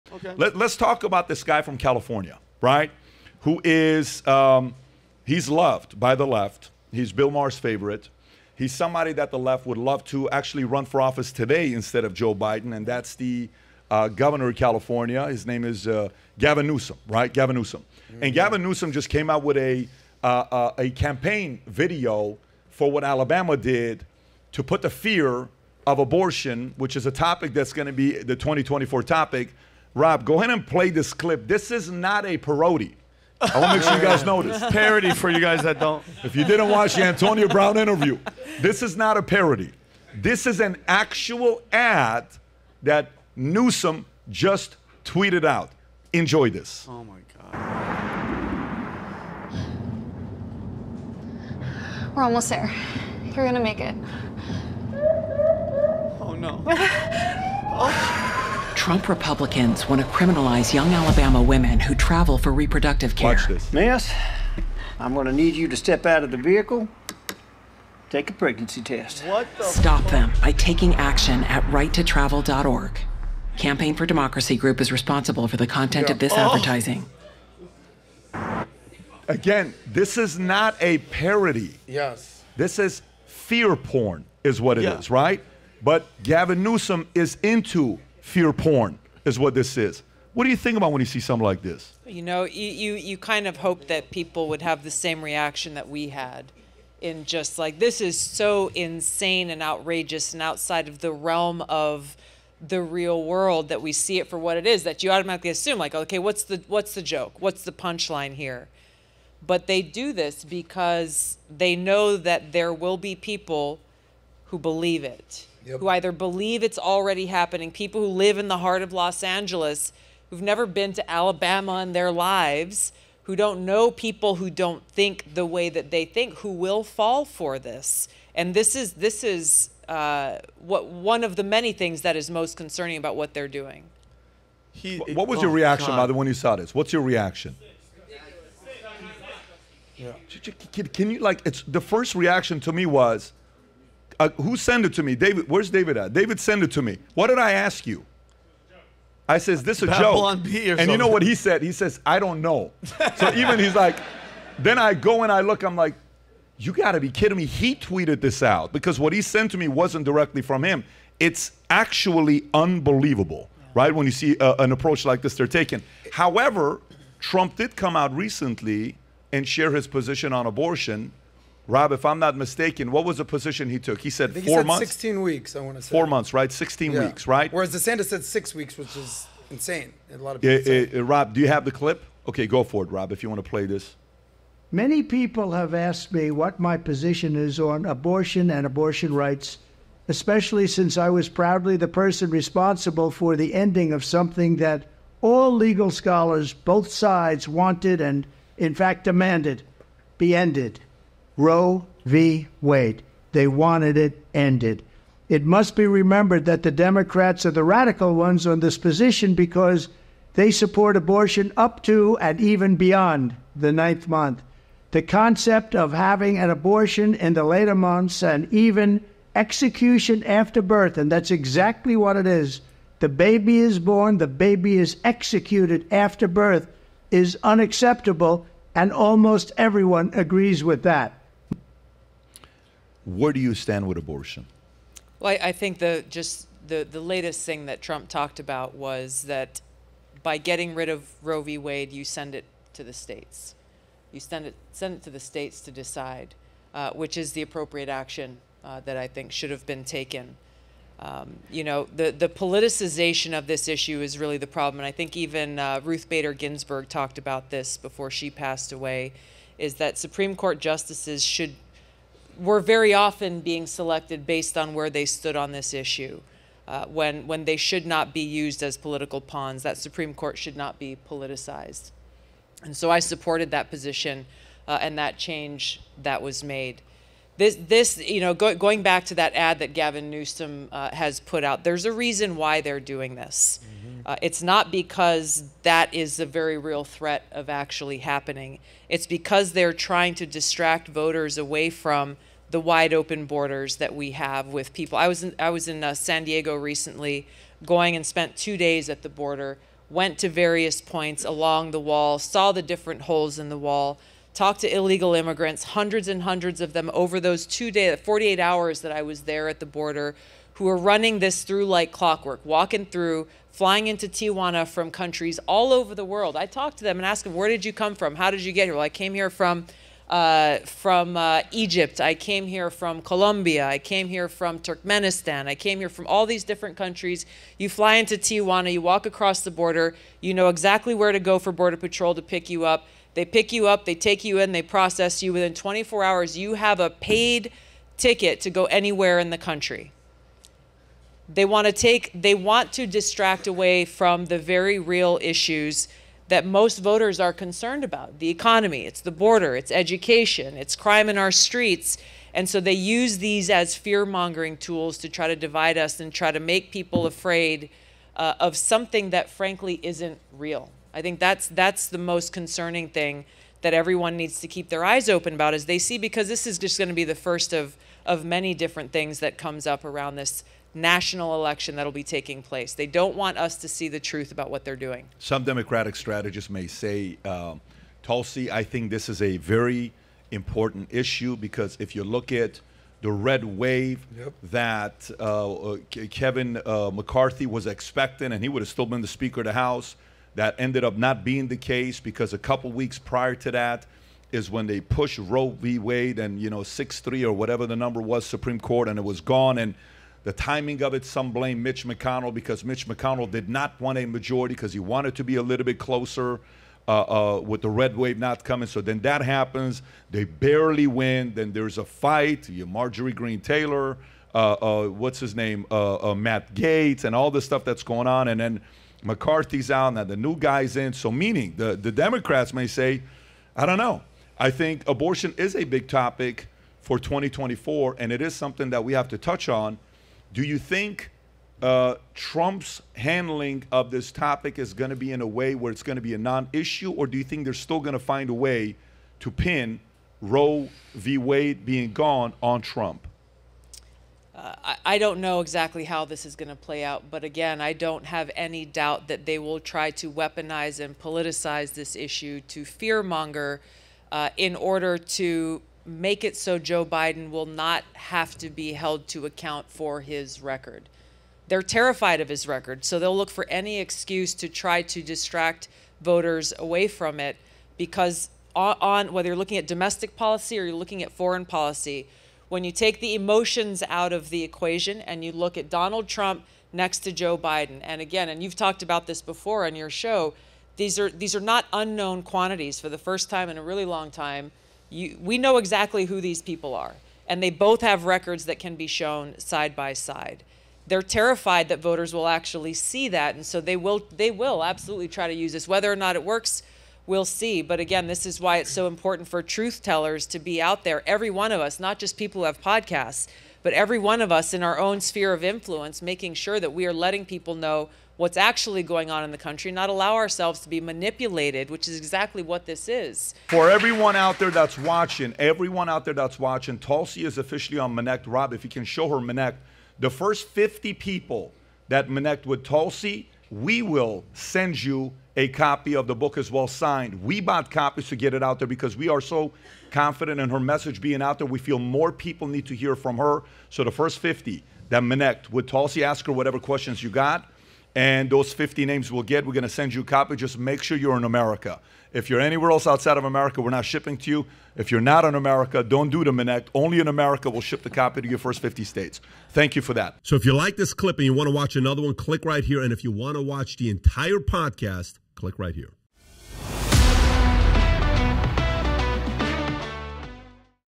“Fear Mongering Tools”- Gavin Newsom's Abortion Message Is Fear Porn For Californians
Gavin Newsom’s recent abortion campaign video appears aimed at instilling fear in Californians, presenting an extreme perspective on the nation abortion landscape.

Join "The Minnect League Championship" to win a night of dinner & cigars with Patrick Bet-David

Register to win a Valuetainment Boss Set (valued at over $350)